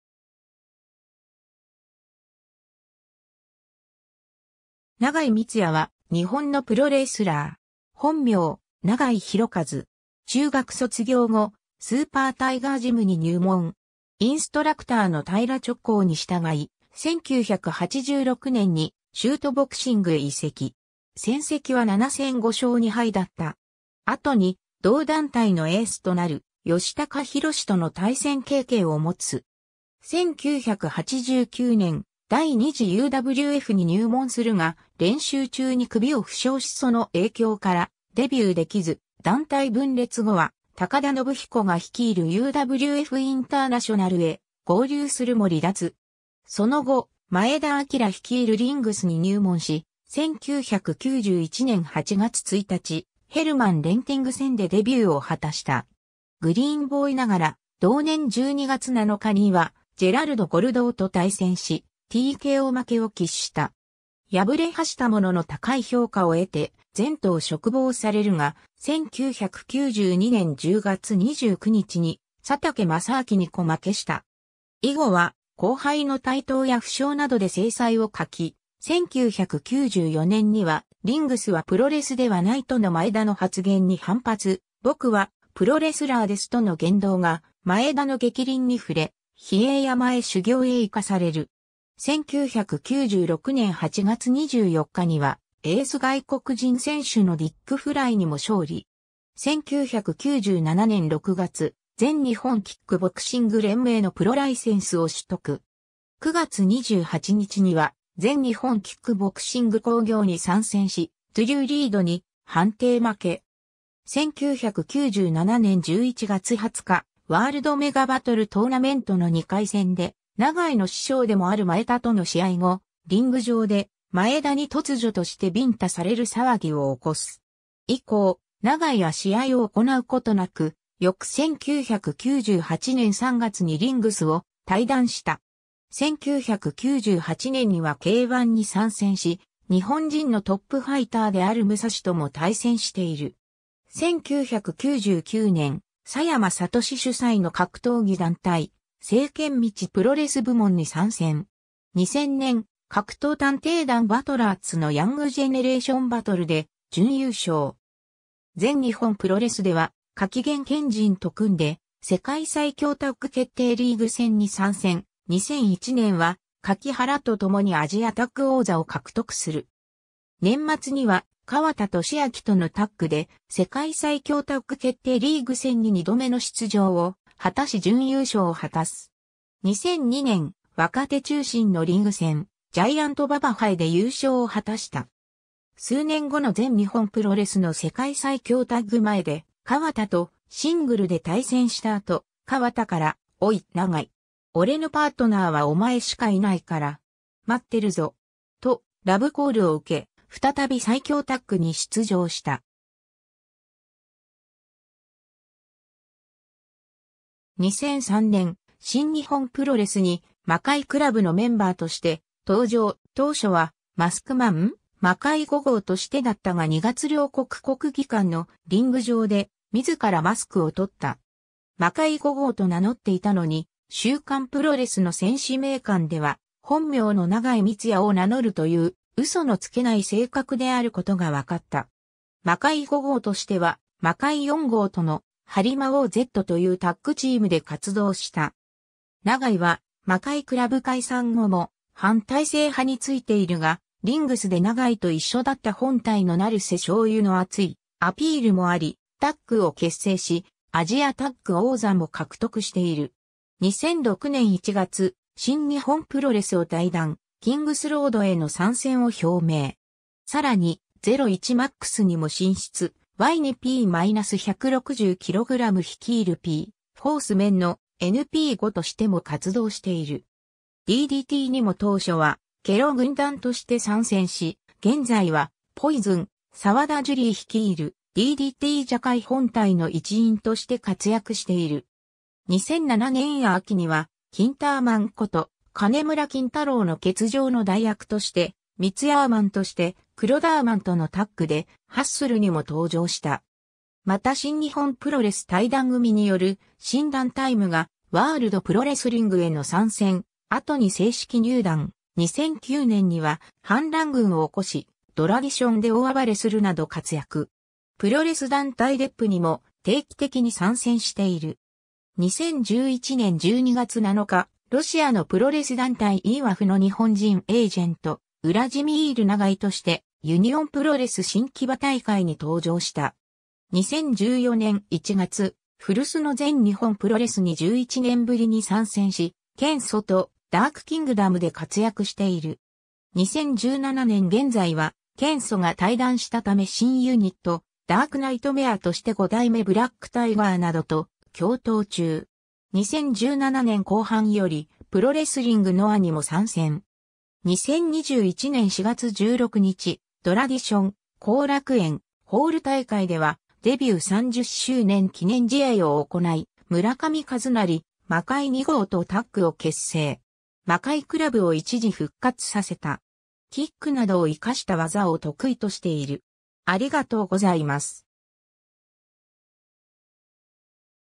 長井満也
長井 満也（ながい みつや、1968年11月10日 - ）は、日本のプロレスラー。
本名：長井 弘和（ながい ひろかず）。
 中学卒業後、スーパータイガージムに入門。
インストラクターの平直行に従い、1986年にシュートボクシングへ移籍。
戦績は7戦5勝2敗だった。
後に同団体のエースとなる吉鷹弘との対戦経験を持つ。
 1989年、第2次UWFに入門するが、練習中に首を負傷しその影響からデビュー出来ず、団体分裂後は高田延彦が率いるUWFインターナショナルへ合流するも離脱。
その後、前田日明率いるリングスに入門し、1991年8月1日、ヘルマン・レンティング戦でデビューを果たした。
グリーンボーイながら同年12月7日にはジェラルド・ゴルドーと対戦しTKO負けを喫した。
敗れはしたものの高い評価を得て前途を嘱望されるが、1992年10月29日に佐竹雅昭にKO負けした。
以後は後輩の台頭や負傷などで精彩を欠き、1994年には「リングスはプロレスではない」との前田の発言に反発、「僕はプロレスラーです」との言動が前田の逆鱗に触れ、比叡山へ修行へ行かされる。
1996年8月24日にはエース外国人選手のディック・フライにも勝利。
 1997年6月、全日本キックボクシング連盟のプロライセンスを取得。
9月28日には全日本キックボクシング興行に参戦し、ドゥリュー・リードに判定負け。
 1997年11月20日、WORLD MEGA BATTLE TORNAMENTの2回戦で長井の師匠でもある前田との試合後、リング上で前田に突如としてビンタされる騒ぎを起こす。
以降、長井は試合を行うことなく翌1998年3月にリングスを退団した。
 1998年にはK-1に参戦し、日本人のトップファイターである武蔵とも対戦している。
1999年、佐山聡主宰の格闘技団体「掣圏道」プロレス部門に参戦。
 2000年、格闘探偵団バトラーツのヤング・ジェネレーション・バトルで準優勝。
全日本プロレスでは、垣原賢人と組んで世界最強タッグ決定リーグ戦に参戦、2001年は、垣原とともにアジアタッグ王座を獲得する。
年末には川田利明とのタッグで世界最強タッグ決定リーグ戦に2度目の出場を果たし準優勝を果たす。
2002年、若手中心のリーグ戦「ジャイアント馬場杯」で優勝を果たした。
数年後の全日本プロレスの世界最強タッグ前で川田とシングルで対戦した後、川田から「おいっ長井。オレのパートナーはお前しかいないから。待ってるぞ!」とラブコールを受け、再び最強タッグに出場した。
 2003年、新日本プロレスに魔界倶楽部のメンバーとして登場、当初はマスクマン「魔界5号」としてだったが2月両国国技館のリング上で自らマスクを取った。
魔界5号と名乗っていたのに週刊プロレスの選手名鑑では「本名の長井満也」を名乗ると言う嘘のつけない性格であることがわかった。
魔界5号としては、魔界4号（正体は柴田勝頼）との「破悧魔王'Z（はりまおうず）」というタッグチームで活動した。
長井は魔界倶楽部解散後も反体制派についているが、リングスで長井と一緒だった本隊の成瀬昌由の熱いアピールもあり、タッグを結成しアジア・タッグ王座も獲得している。
 2006年1月、新日本プロレスを退団、キングスロードへの参戦を表明。
さらにZERO1-MAXにも進出、Y2P-160kg率いるP-Force Menの「NP5」としても活動している。
DDTにも当初はケロ軍団（田中秀和、後藤達俊、長井）として参戦し、現在はポイズン澤田JULIE率いるDDT蛇界本隊の一員として活躍している。
 2007年秋にはキンターマンこと金村キンタローの欠場の代役としてミツヤーマンとしてクロダーマン（黒田哲広）とのタッグでハッスルにも登場した。
 また新日本プロレス退団組による新団体無我ワールド・プロレスリング（現・ドラディション）への参戦、後に正式入団、2009年には反乱軍を起こしドラディションで大暴れするなど活躍。
プロレス団体DEPにも定期的に参戦している。
 2011年12月7日、ロシアのプロレス団体IWFの日本人エージェント、ウラジミール長井としてユニオンプロレス新木場大会に登場した。...